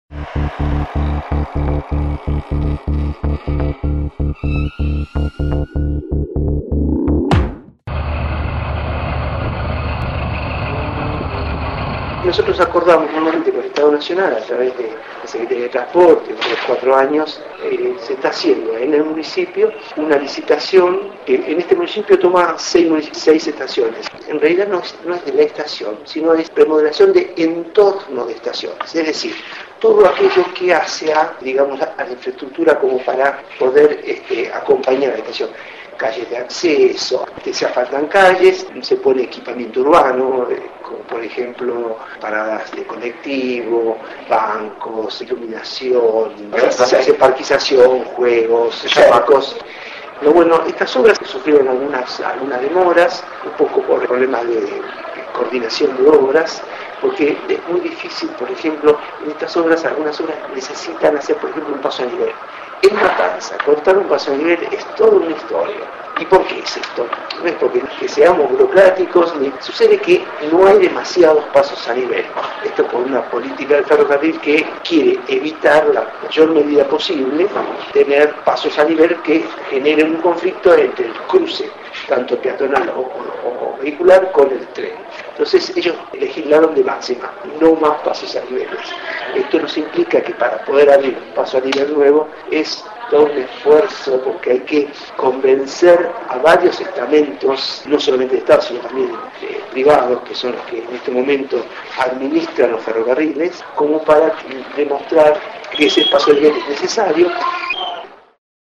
Finalizaron las obras en estaciones ferroviarias de La Matanza
En el marco de un programa de mejoramiento de la seguridad vial, está a punto de concluir la renovación de cinco estaciones de la línea Belgrano y una de la Roca, así como varios pasos a nivel de distintas localidades. Ahora, apuntan contra los peligrosos.